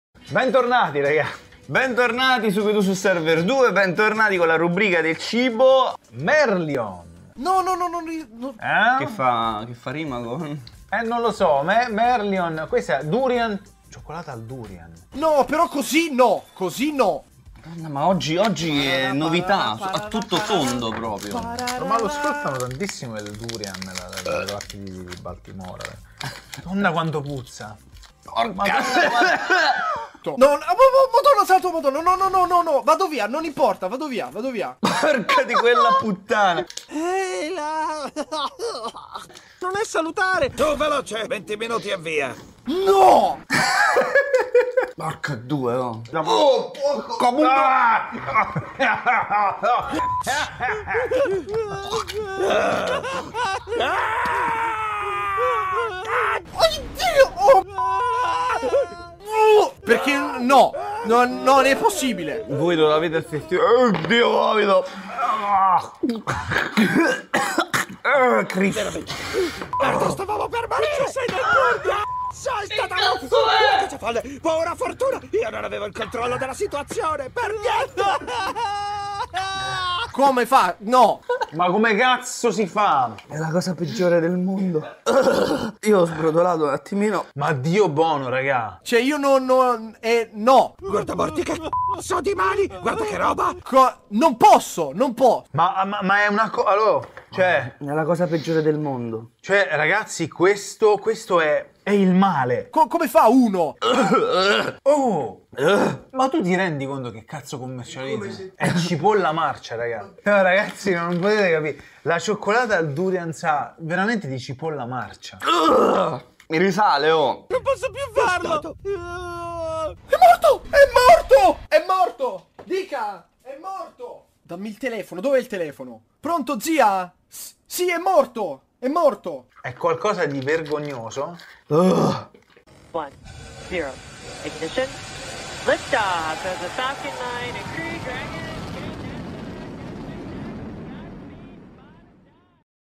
QDSS, YOTOBI, RICK AND MORTY, abbiamo vomitato col Durian #19
CARTONE DI INTRATTENIMENTO E "CIBO"
Capitolo 19 del cartoon: Cartoon Crossover, Divulgatori, Youtubers e Intrattenitori.
PS questa scena non è mai avvenuta nella realtà, ed è un peccato :)